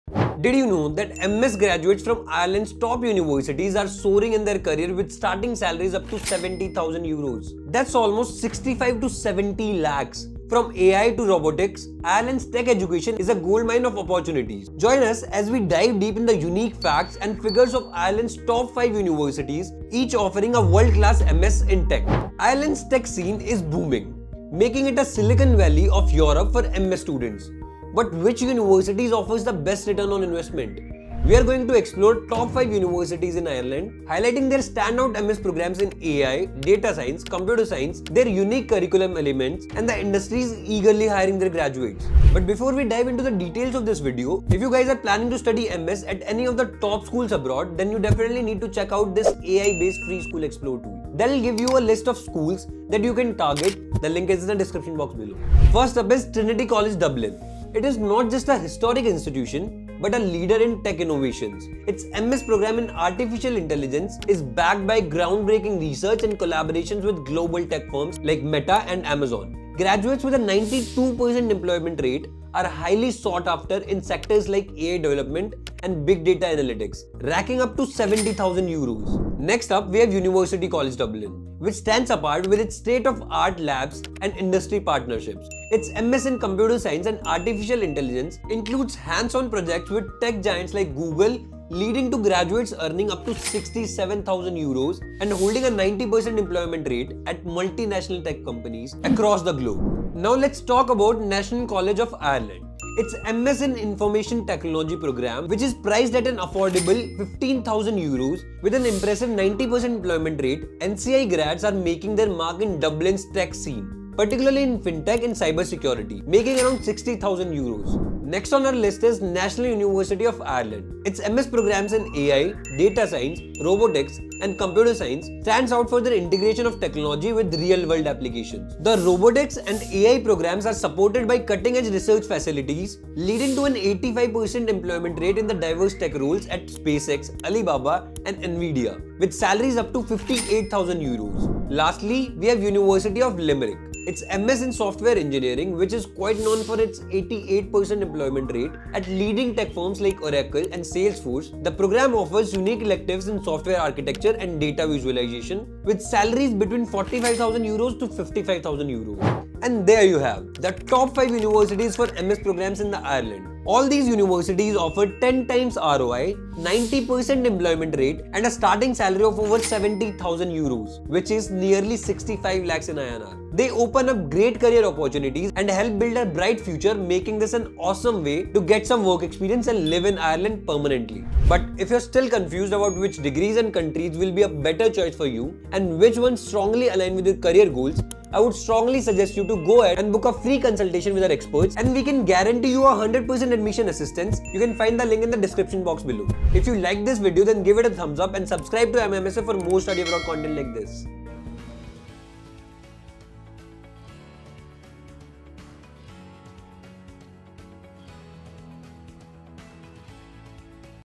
Top 5 Ireland Universities for Masters in 2024
👉 Take our FREE School Explore Tool, to understand your strengths and weaknesses and know which schools you can target: 🔗

🚀 Are you dreaming of a high-flying tech career with an MS in Ireland? 🎓 You're in the right place! In this video, we unveil the top 5 Ireland universities for masters, especially in tech fields like AI, Data Science, and Robotics. 🤖💡

🔍 What you'll find in this video:

➡️ Trinity College Dublin: A historic leader in tech innovation with an impressive 92% employment rate and starting salaries up to €70,000 in AI and big data analytics. 🏛️👨‍💻
➡️ University College Dublin: Known for its cutting-edge labs and partnerships with tech giants, its graduates earn up to €67,000 with a 90% employment rate. 🎓🌐
➡️ National College of Ireland: Affordable yet impactful, with a €62,000 starting salary in Dublin's tech scene, especially in fintech and cybersecurity. 💻🔒
➡️ National University of Ireland: Offers diverse MS programs with a strong practical focus, leading to an 85% employment rate and salaries up to €58,000. 🌟🤖
➡️ University of Limerick: Recognized for its MS in Software Engineering, boasting an 88% employment rate and salaries between €45,000 and €55,000. 🖥️🎓

🔔 Turn on the bell notification for updates on our latest videos!

👇 Timestamps:

00:00 - Introduction: MS in Ireland Overview
0:56 - Trinity College Dublin: AI and Big Data Analytics
01:21 - University College Dublin: Computer Science and AI
01:40 - National College of Ireland: Information Technology Focus
01:58 - National University of Ireland: AI, Data Science, and Robotics
02:22 - University of Limerick: Software Engineering Expertise
02:40 - Conclusion: ROI and Career Opportunities